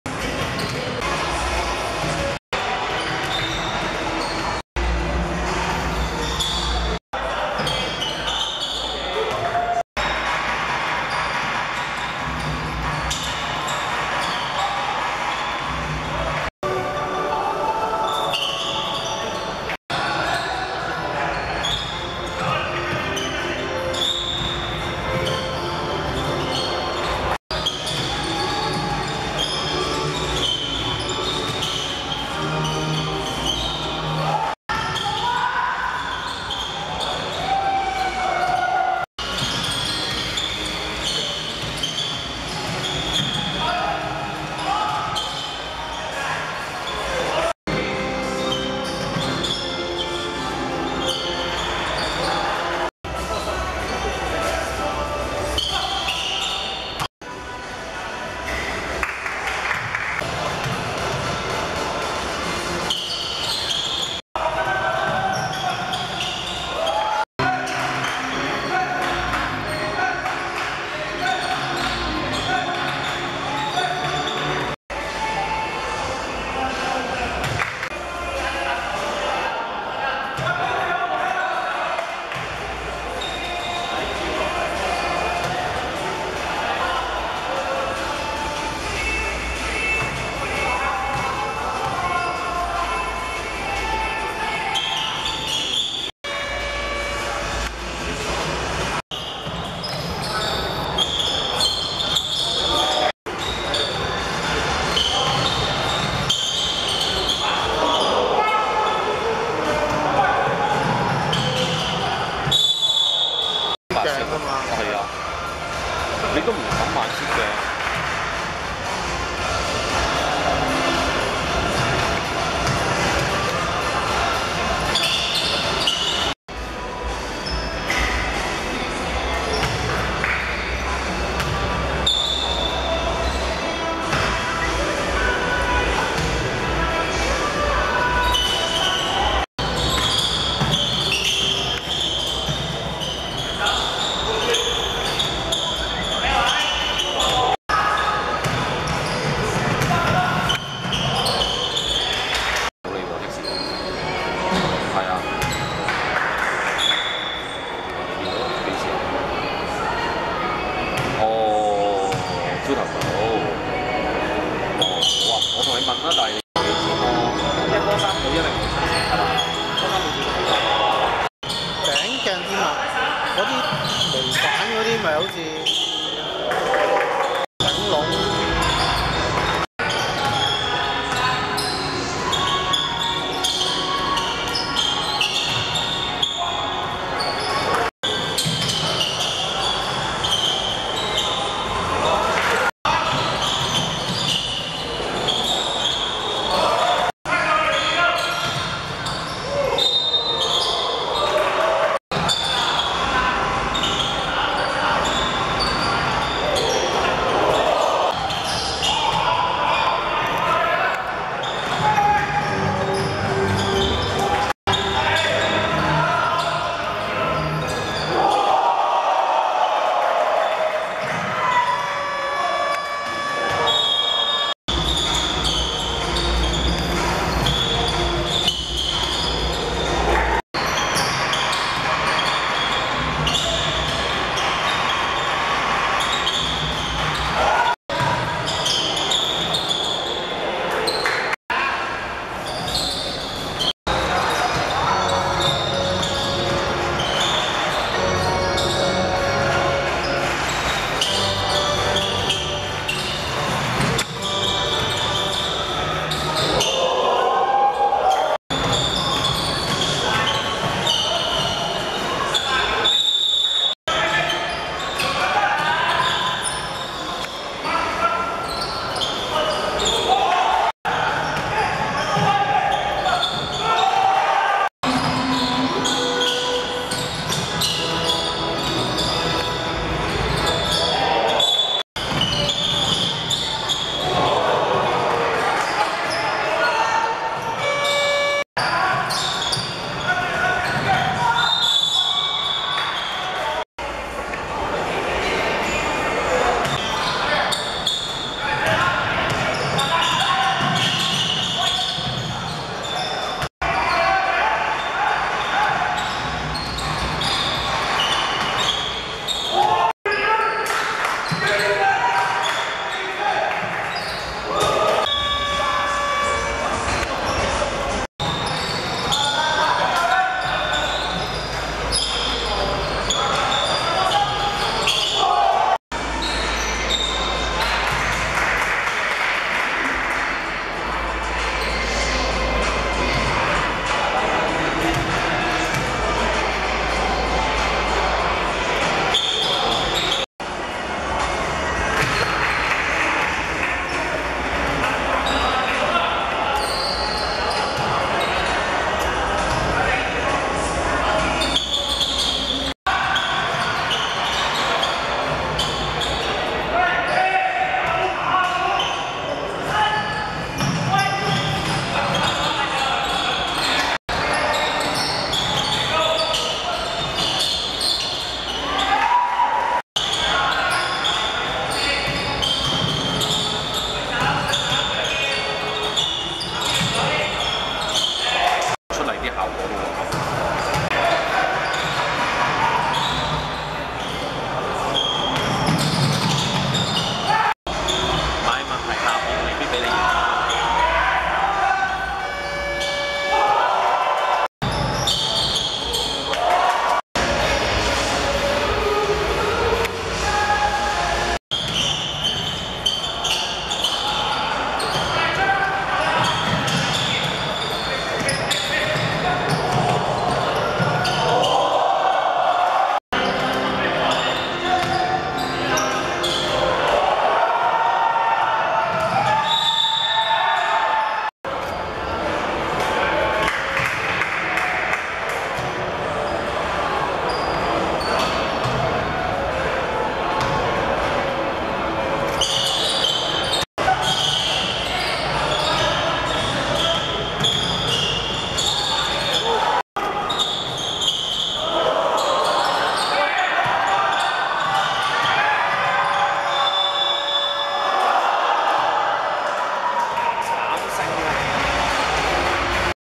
嶺大vs理大(2015.8.26.Jordan Take Flight Invitational大專籃球賽)精華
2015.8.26.修頓塲館 15:30 嶺大(白) 54 : 56 理大(紅)